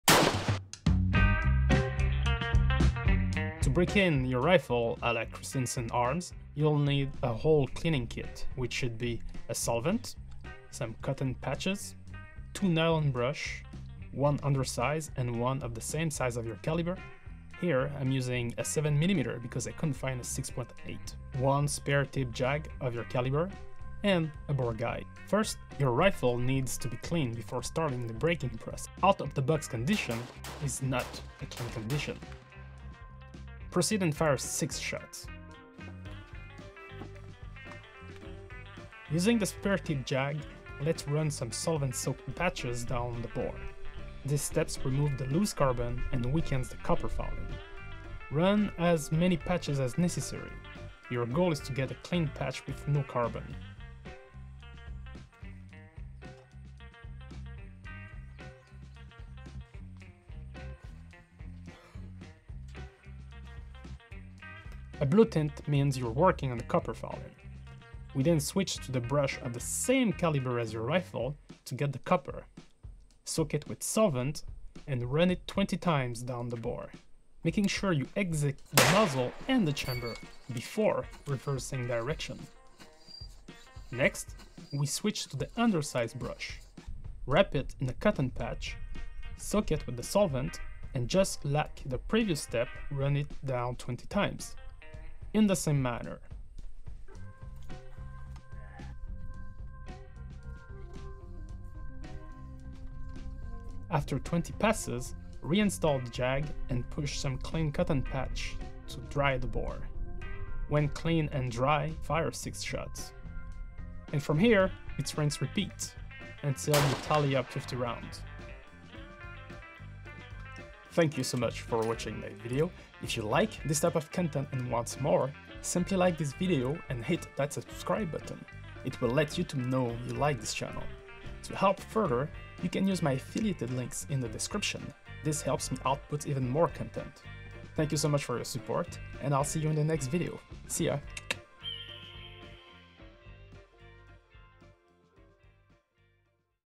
Mastering the Art: Barrel Break-in the Christensen Arms Way
In this video, we'll be following the Christensen Arms Barrel Break-in Procedure. This procedure is used to improve the accuracy and reliability of your firearm.

 I call BS when you see a brand-new rifle, and the makers claim it doesn't require any break-in. (Unless they've done it for you) Break-in your barrel is up to you, but the benefits are evident. And many barrel manufacturers recommend it. Everyone has their preferred method, But Christensen Arms' procedure is clearly thorough and quite an investment... 50 rounds!